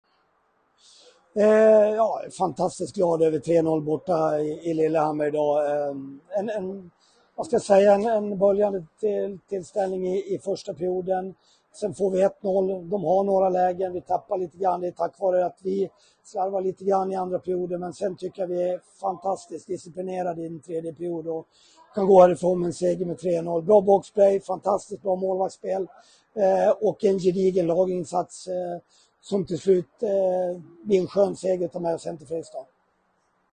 Fredrik etter seier mot Lillehammer
Hør hva Fredrik hadde å si etter 3-0 seier borte mot Lillehammer.
110124